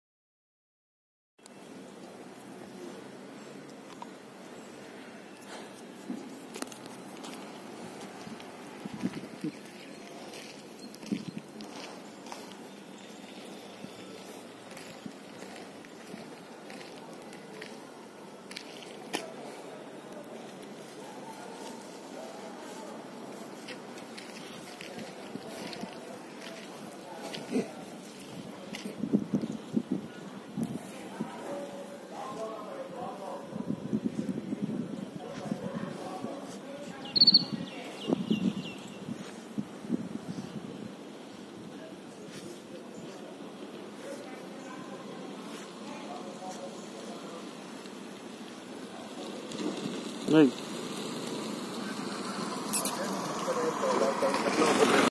👍 Best Funny Dogs 🐶 - Intelligent Dog.
This Dog is funny but intelligent. It wants to hide his treat inside the construction sand so he can come back and eat.

Indian street dog or Indian pariah dogs also known as INDog. They are intelligent dogs with excellent social skills and intuitive abilities. We have three street dog who guard our area.
Indian strays are naturally alert and tend to guard the streets that they live on. If you show one of the strays on your street some affection or begin feeding them, they will be your guards night and day. They bark on the slightest suspicion to alert other dogs in the area and humans as well.